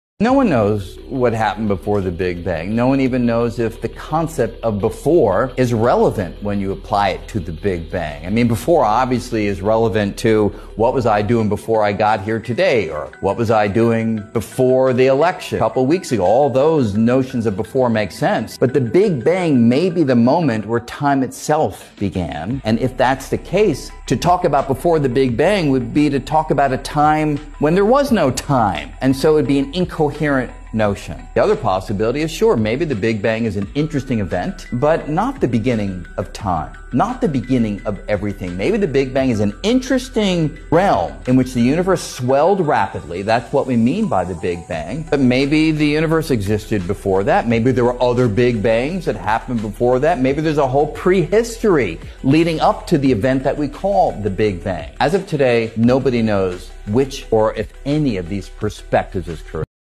🌀What came before the Big Bang?🤯Time may have started there...
What happened before the Big Bang? 🌀 Was there even a "before"? 🕰️ Brian Greene explains how the Big Bang might be the origin of time itself—or just a chapter in a much deeper cosmic history. Could there have been multiple Big Bangs? Could the universe have a prehistory we can’t yet imagine? This mind-bending clip challenges our understanding of time, space, and the origin of everything.